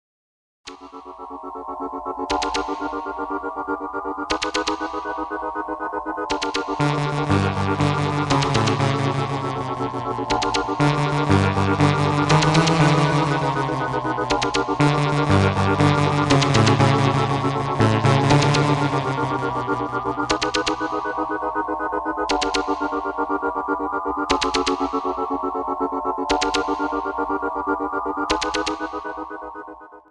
The ONM Nintendo music quiz - Track 14 (Hard)
Guess which Nintendo game this track is from.